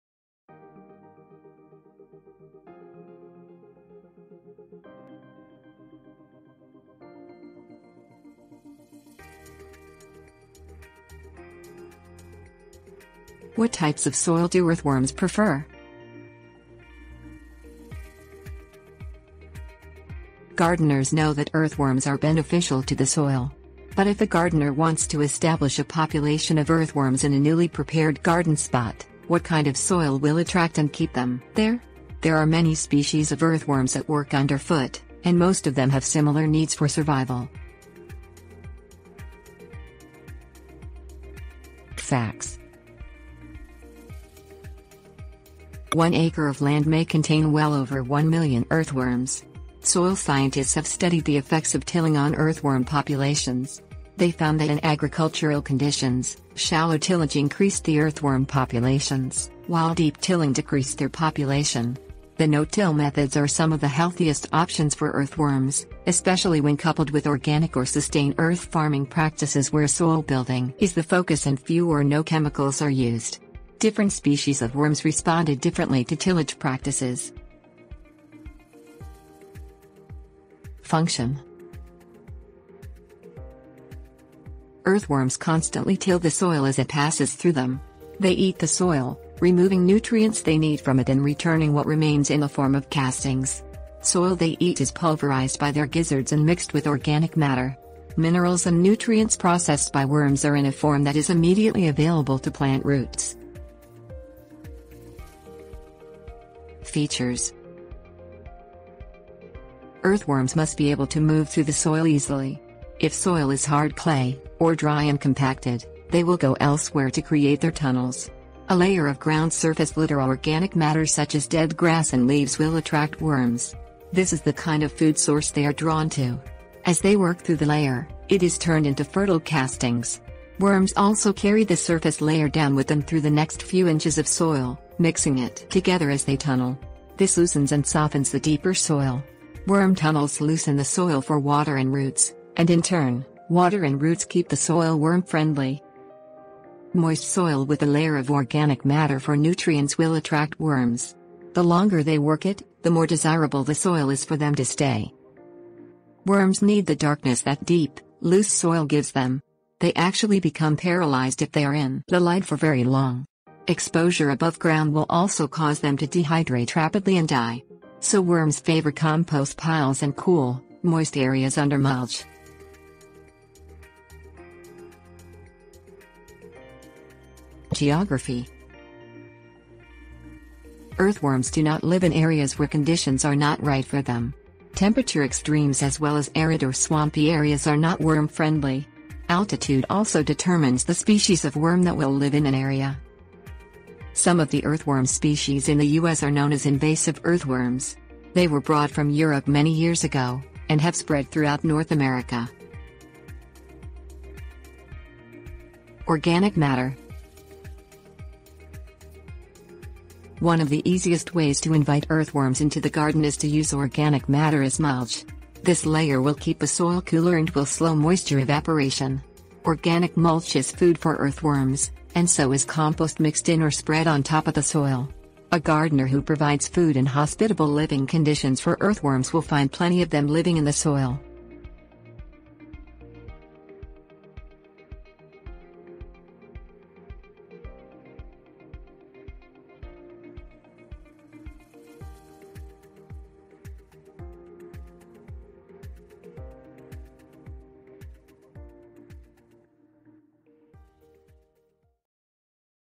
What Types of Soil Do Earthworms Prefer?
What Types of Soil Do Earthworms Prefer?. Gardeners know that earthworms are beneficial to the soil. But if a gardener wants to establish a population of earthworms in a newly prepared garden spot, what kind of soil will attract and keep them there? There are many species of earthworms at work underfoot, and most of them have similar needs for...

Table of contents What Types of Soil Do Earthworms Prefer?
Facts 00:40
Function 01:19
Features 01:45
Geography 02:56
Organic Matter 03:25